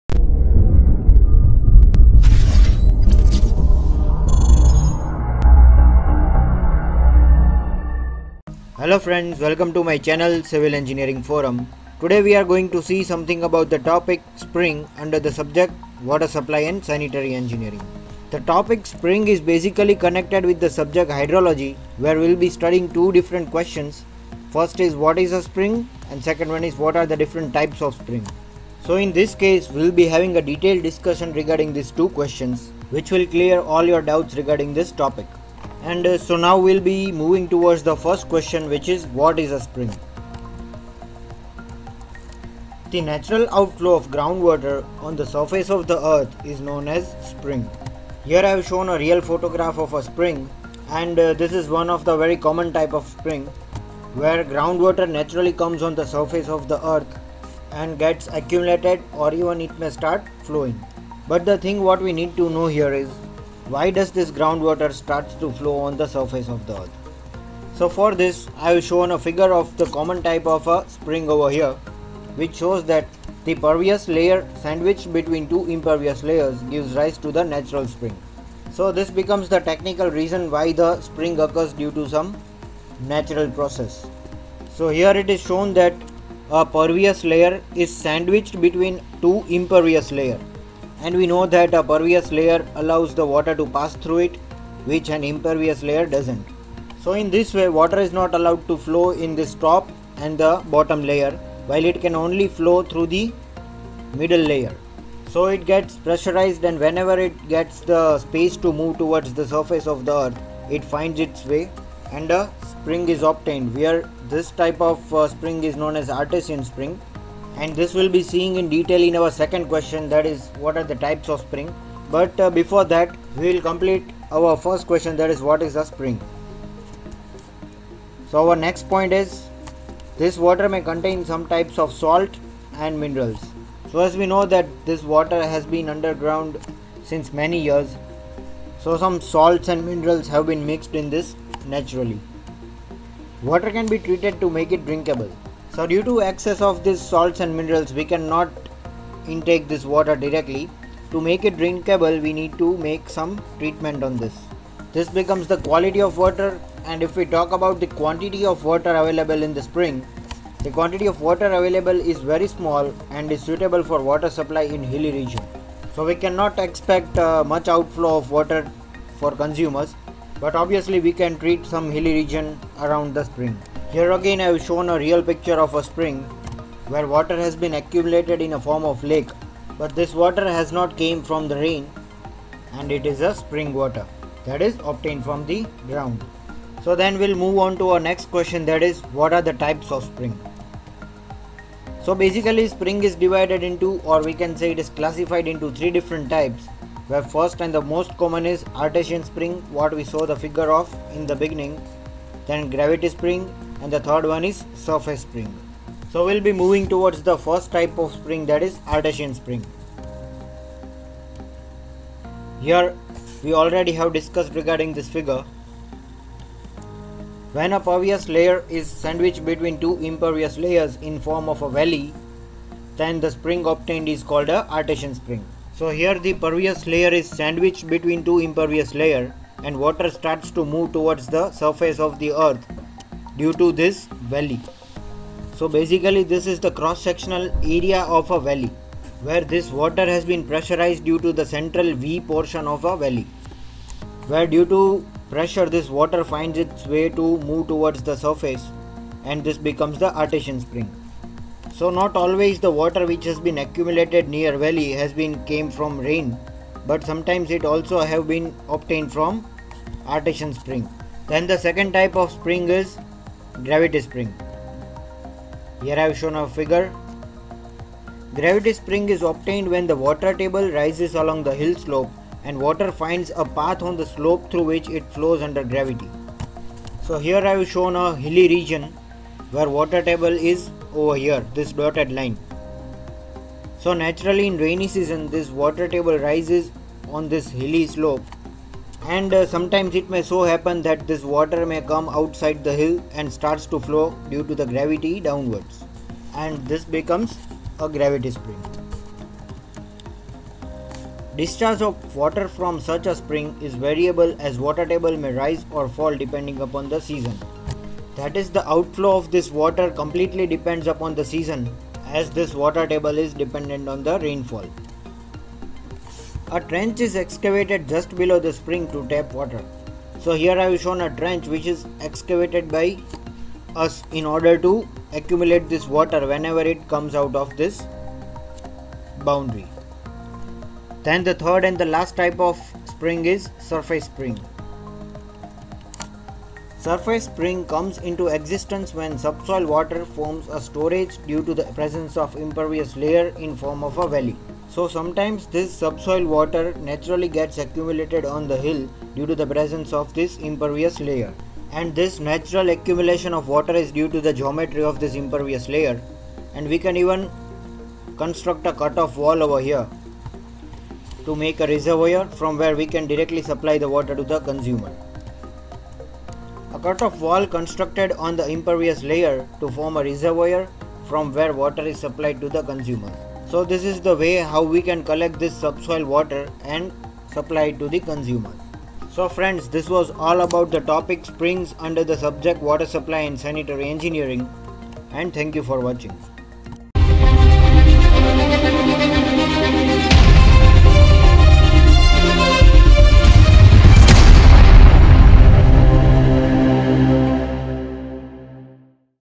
What is a spring ?
In this video i have shown what is a Spring under the subject Water Supply and Sanitary Engineering.
Types of spring are also explained with photographs namely artesian spring, surface spring and gravity spring.